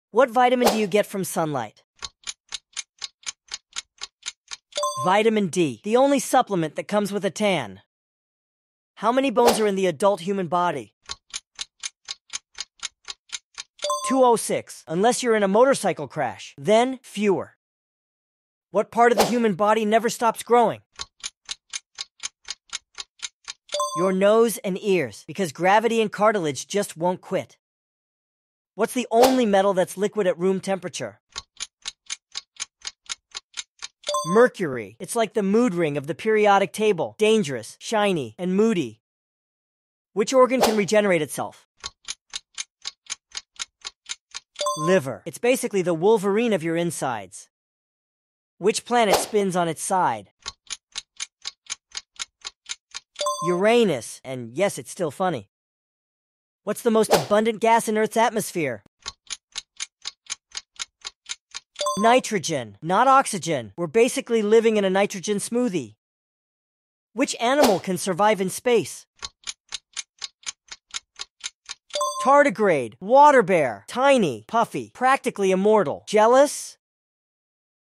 Think You’re Smarter Than a 5th Grader? Try This Science Gauntlet
8 science questions. No choices. No mercy.
Easy to super hard—can you run the table?

Welcome to Quiz Master: Science.
You get 8 questions—no multiple choice, no lifelines, and no excuses.
We start easy. Then medium. Then hard. Then brutal.

This one’s for the science lovers, trivia nerds, and know-it-alls who shout answers at the screen.
If you ace all 8, you might just be one of us.